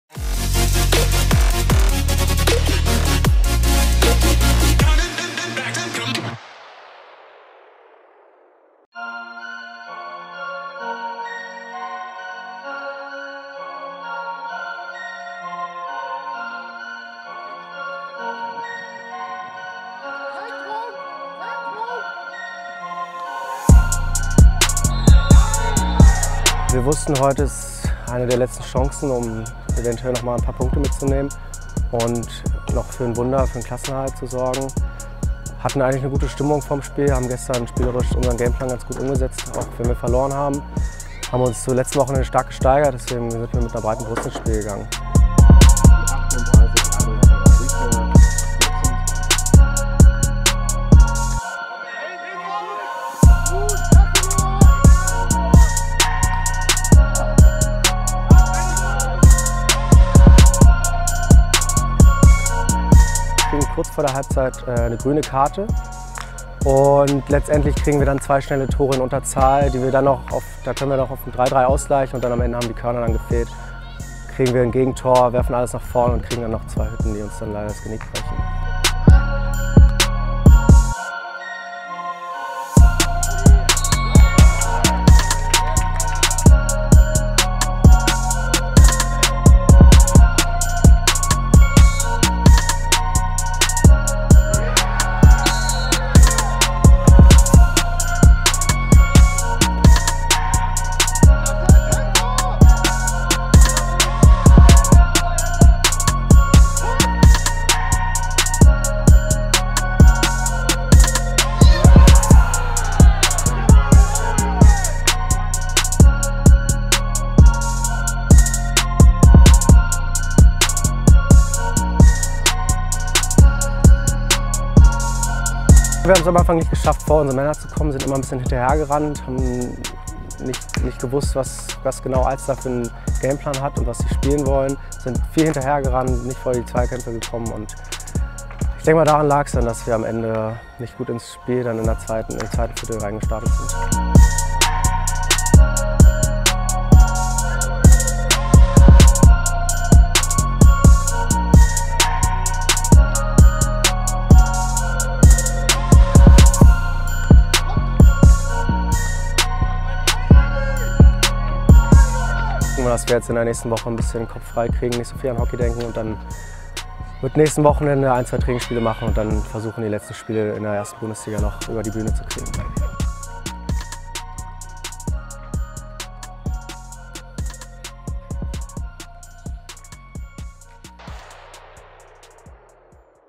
Gegen den Abstieg: TC1899 n Club an der Alster | Hockey Bundesliga
Was für ein emotionales Wochenende für die TC Blau-Weiss Hockey Herren der 1. Bundesliga! Es waren die zwei entscheidenden Spiele, denn hier ging es darum,möglichst viel Abstand zu den Abstiegsplätzen zu erspielen.